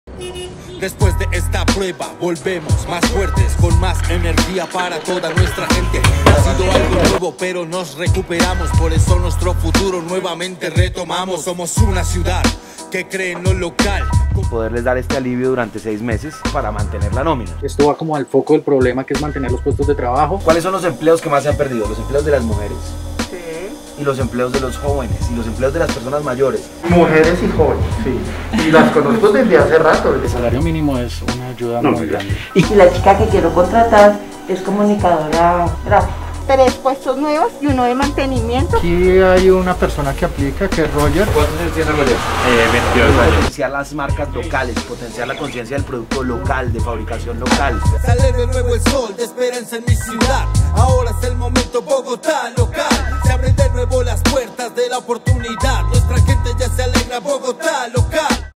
¿Tienes un negocio en tu barrio y necesitas mantener los empleos? Postúlate a EsMicroempresaLocal
¿Tienes un pequeño negocio en tu barrio y necesitas mantener los empleos y/o vincular a jóvenes, mujeres y mayores de 50 años?

Llegó EsMicroempresaLocal para apoyarte con un subsidio a la nómina de 1 salario mínimo mensual, por 6 meses.